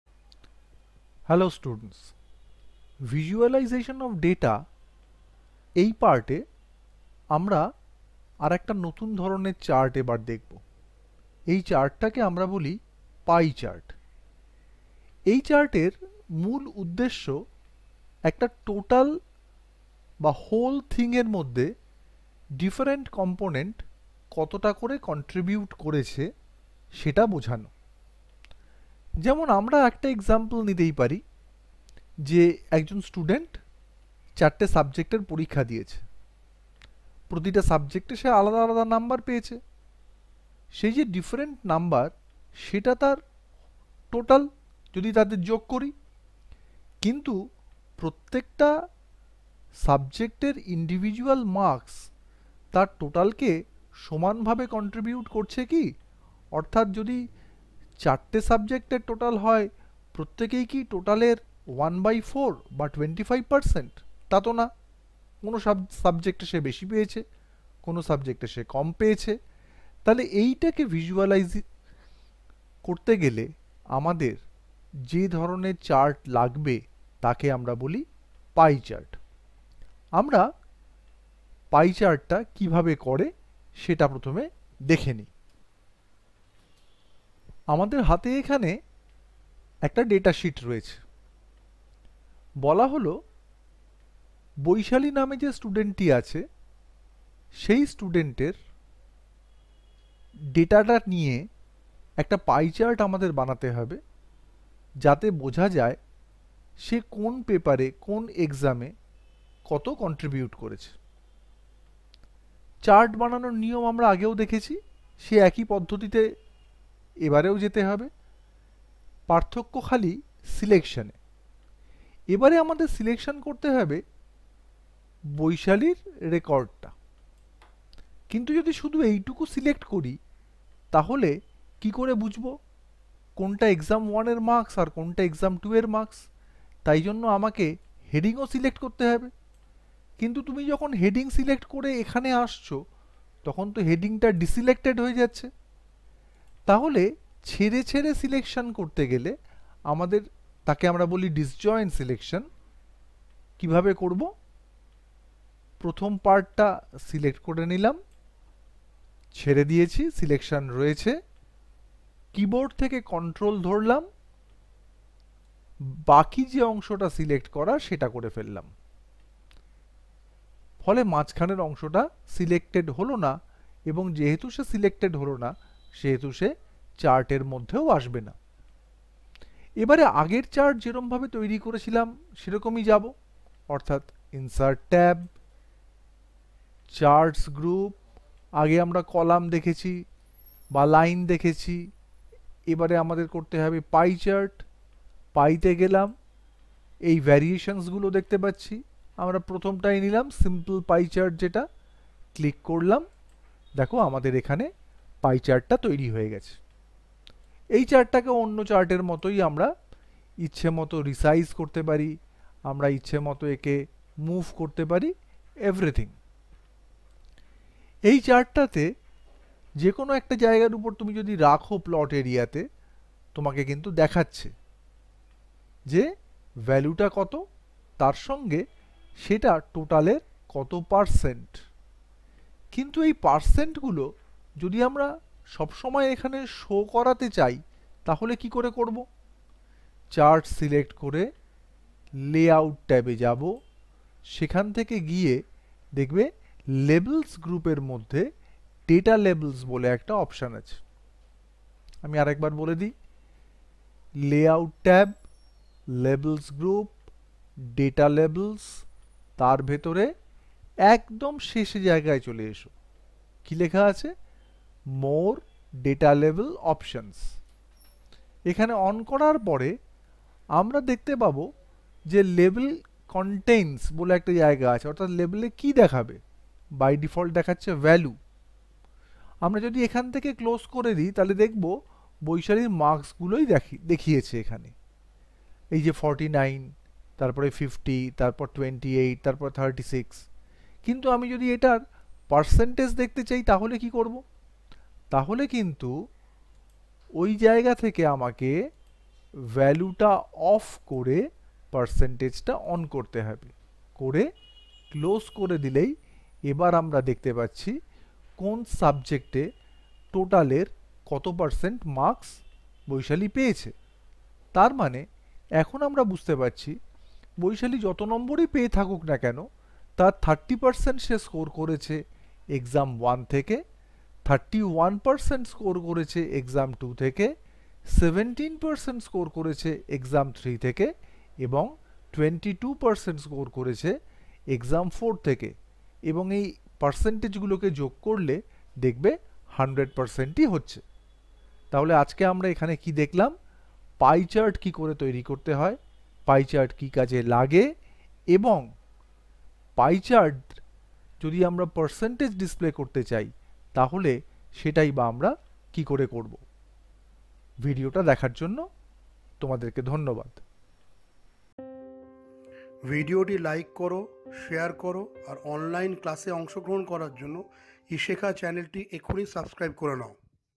Pie Chart/Microsoft Excel 2013/Computer Training Institute Under State Youth Centre
This video demonstrate how to Work with Ms Excel Pie Chart. This is a easy tutorial to demonstrate Work with Ms Excel 2013 Pie Chart for beginners.